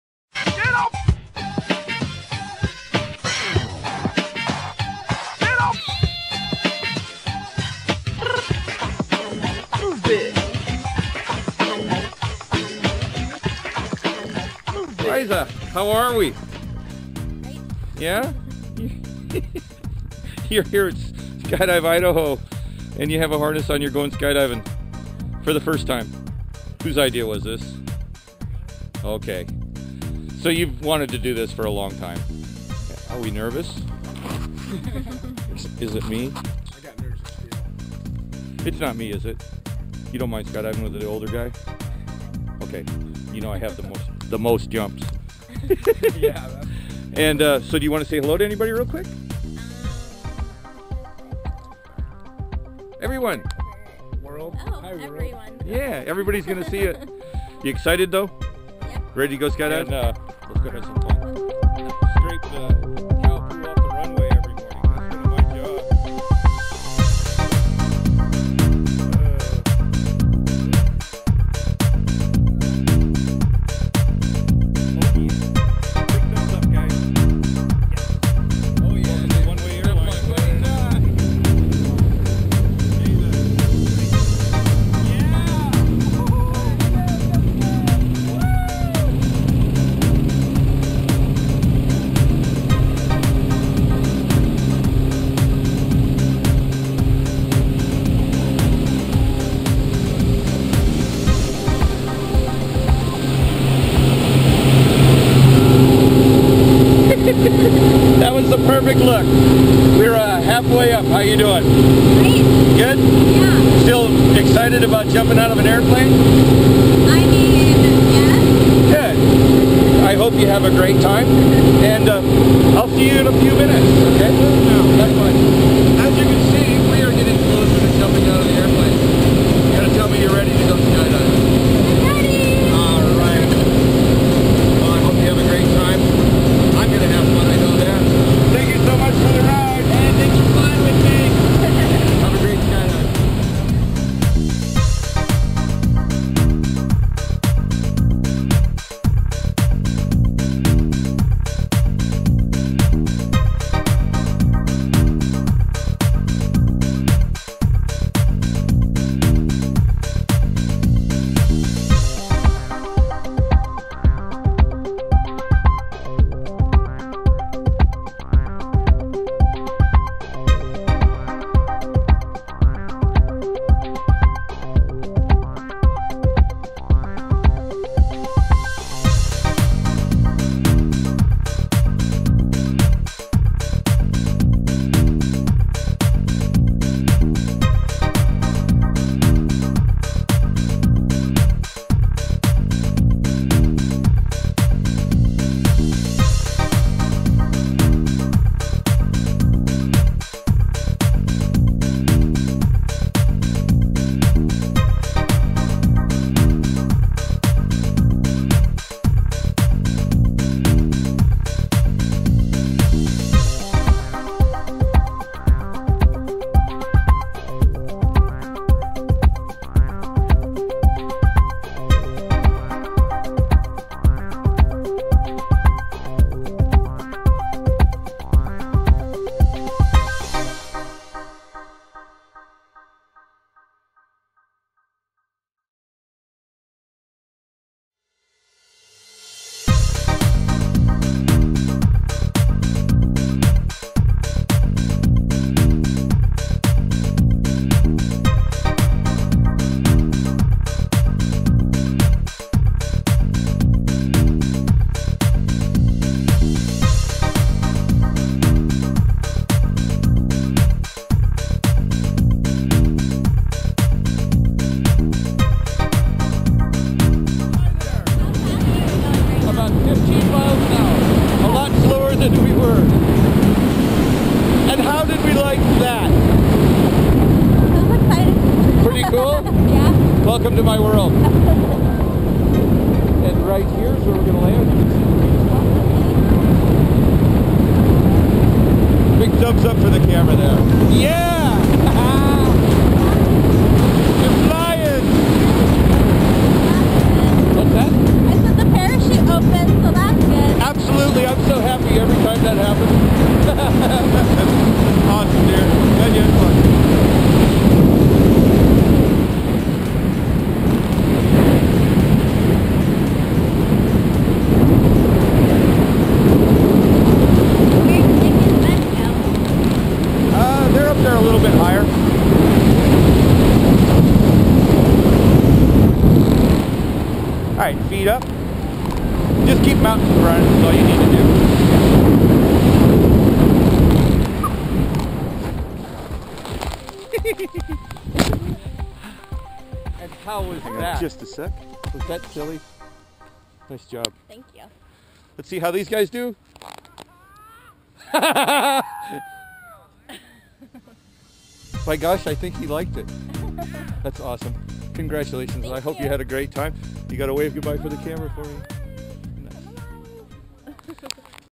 Beise Kamenova's DZONE Tandem skydive at Skydive Idaho!
Beise Kamenova's DZONE Tandem skydive at Skydive Idaho on Friday, October 17, 2014!